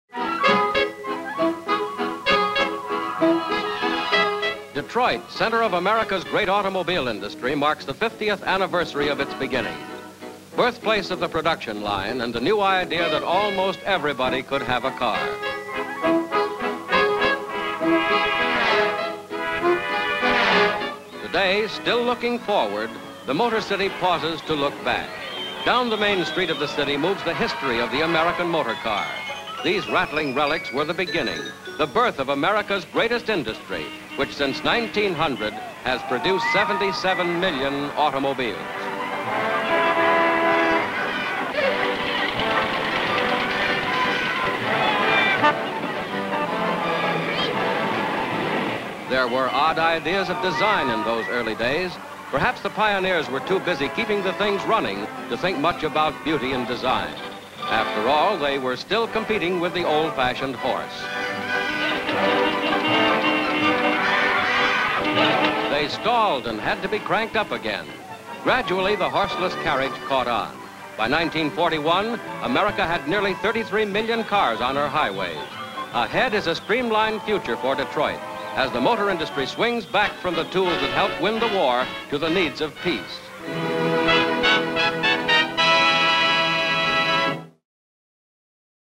50 Years Of American Automobile Industry : Car Parade in Detroit circa 1950 - CharlieDeanArchives
A street parade organized to celebrate 50 years of the automotive industry in Detroit. Vintage cars from the early days of the 20th century take center stage. .